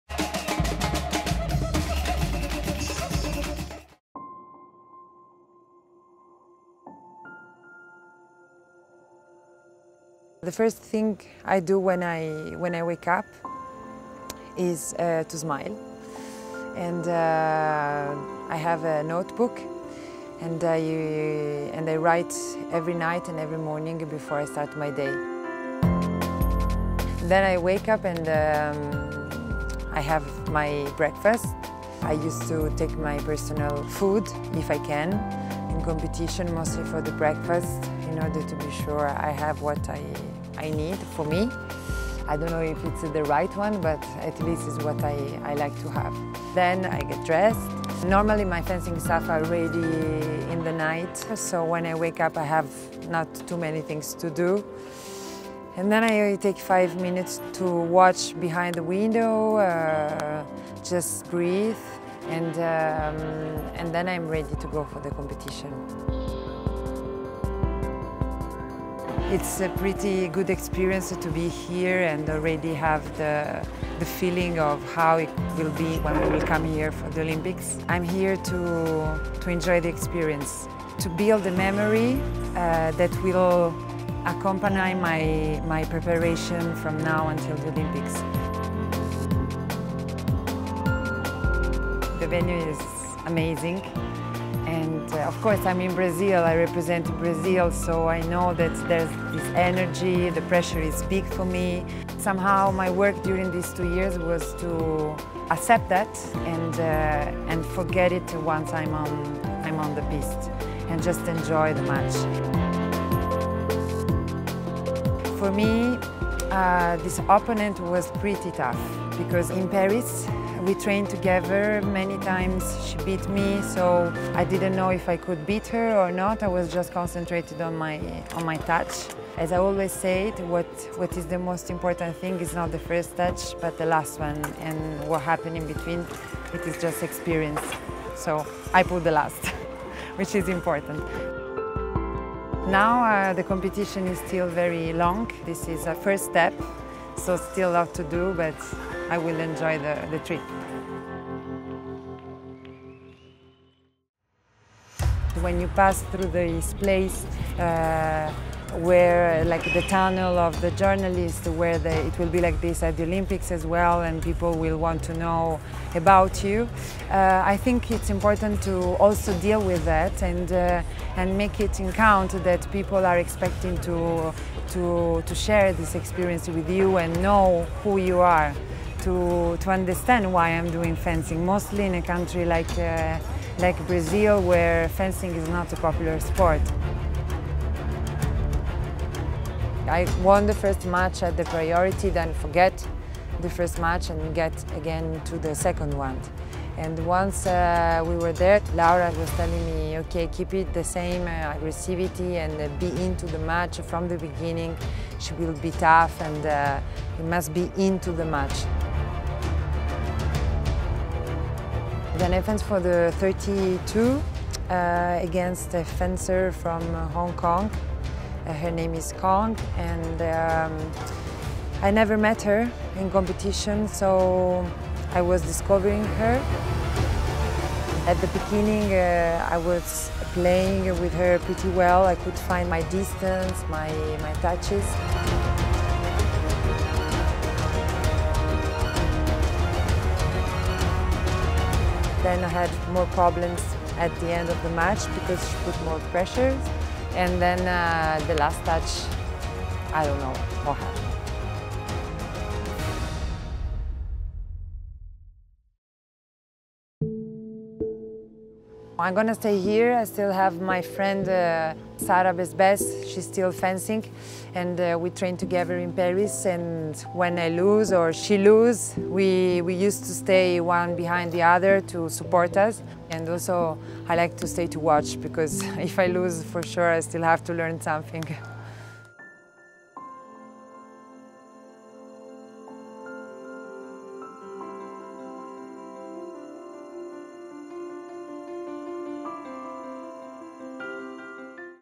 A Day in the Life of Nathalie Moellhausen
How is a day in the life of Nathalie Moellhausen of Brazil. She is competing this August in Rio je Janeiro in women's épée.